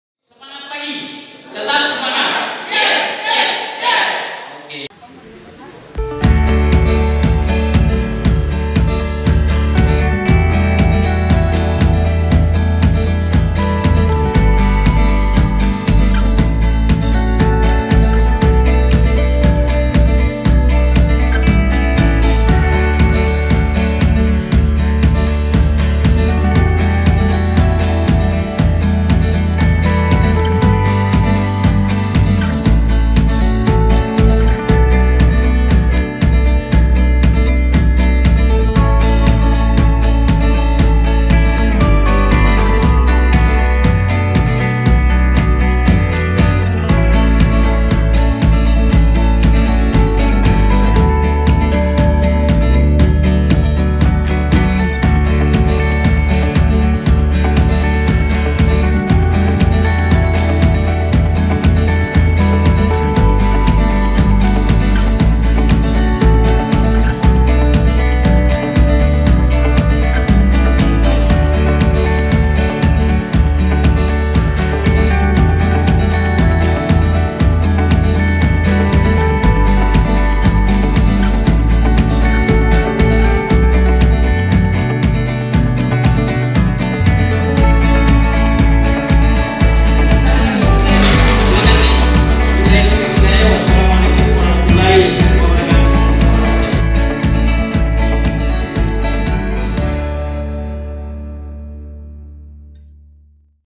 THEMATIC ACADEMY | DISNAKER JENEPONTO
Thematic Academy Eks Migran Batch 2 di Dinas Tenaga Kerja dan Transmigran Kabupaten Jeneponto selama 4 hari mulai tanggal 8-11 Maret 2022.